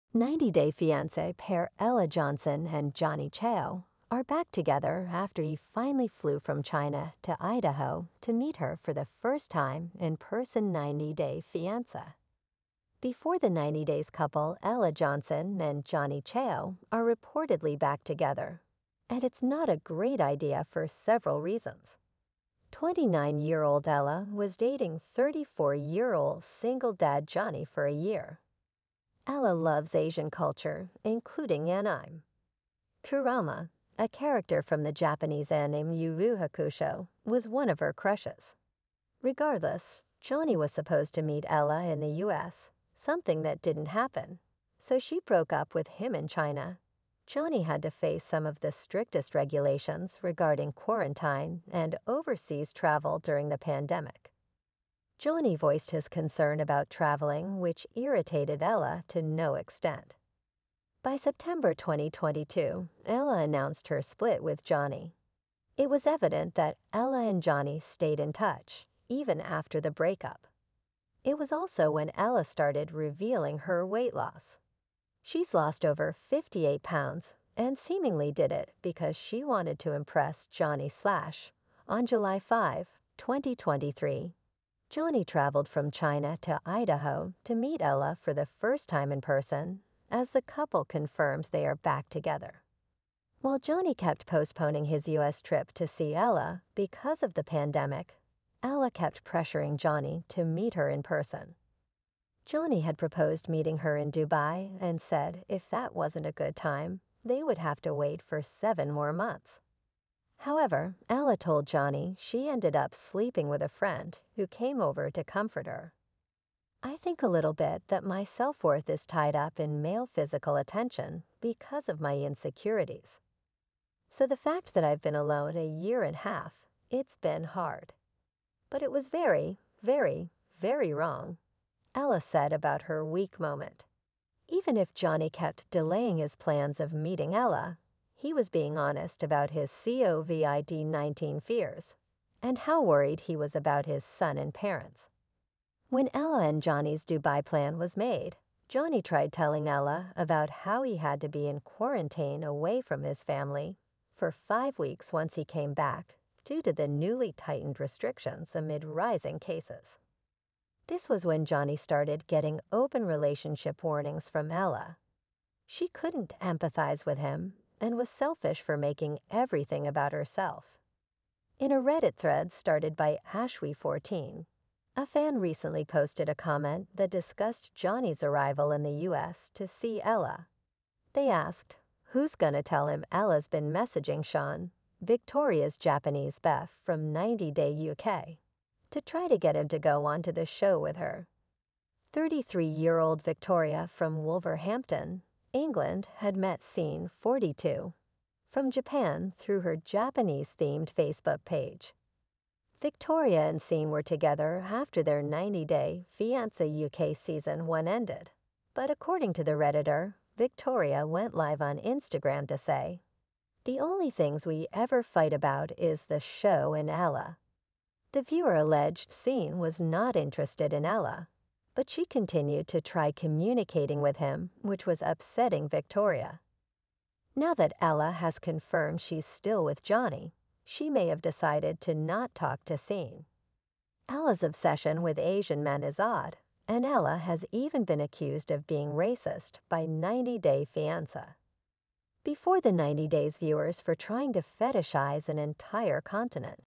90 Day Fiancé's Ella & Johnny Are Making A Serious Mistake By Getting Back Together
90 Day Fiancé's Ella & Johnny Are Making A Serious Mistake By Getting Back Together